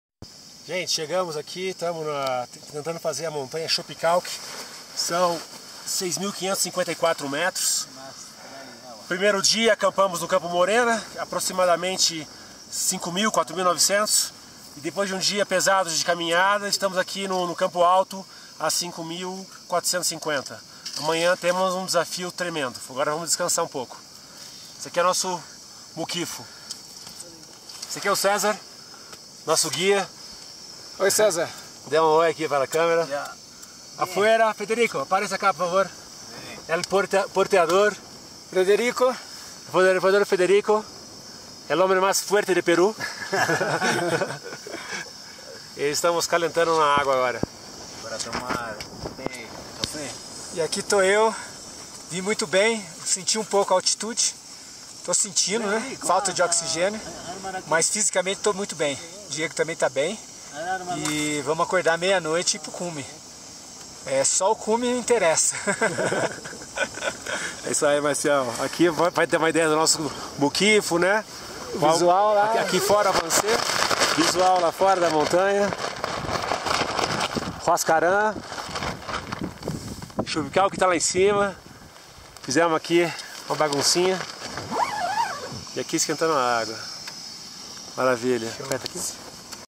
Climbing Nevado Chopicalqui altitude 6300 mts - Huaraz, Peru 2015
We tried to climb "Nevado Chopicalqui" in Peru last year, 2015. We have gone until 6000 meters altitude. This is an small video in advanced camping six hours before the summit attack.